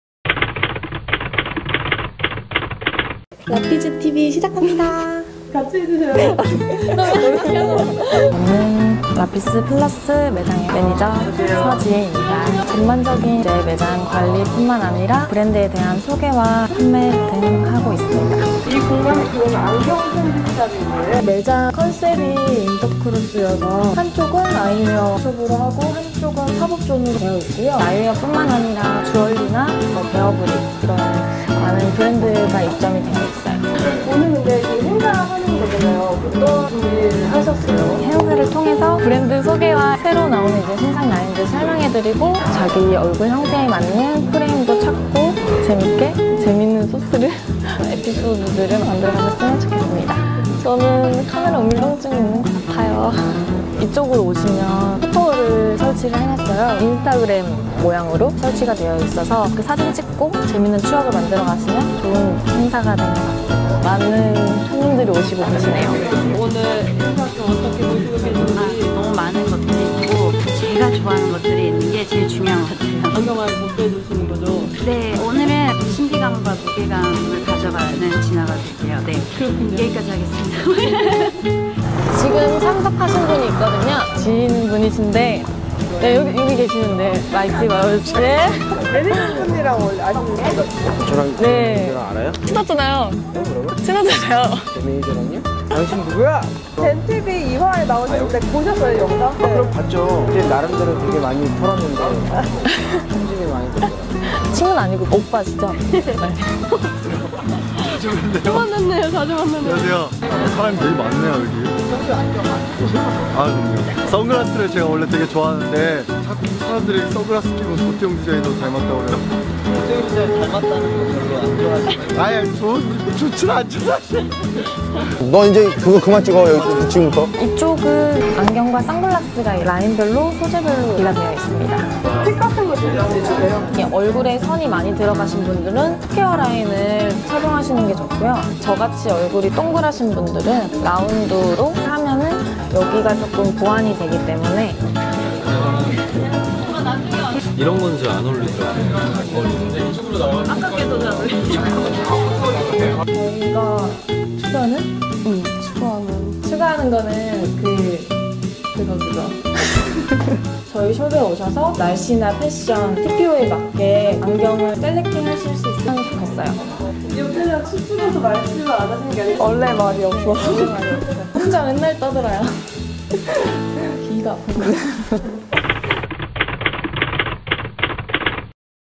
[ENG_브랜드매니저 서지혜] 얼굴에 개성있는 선을 입히다!
배우 이종석, 가수 아이비, 배우 오상진 등 유명 셀럽과 모델들에게 사랑받으며, 스타일리쉬한 국내 안경 브랜드로 자리 잡아가고 있는 '라피스 센시블레'. 가로수길에 위치한 라피스 센시블레 1호점은 안경뿐 아니라 의류, 쥬얼리, 토이 등을 취급해 다양한 볼거리로 발걸음이 아쉽지 않은 공간입니다. 그러한 라피스에서 F/W 캠페인 런칭 파티가 열렸다니 The ICON이 안 가볼 수 없었겠죠? 가수 지나, 쇼리, 언터쳐블, 배우 이영은 등 많은 셀럽이 찾은 그 현장을 ‘라피스’ 티비에서 공개합니다! HD 버튼을 클릭하시면 더 선명한 화질로 만나보실 수 있어요.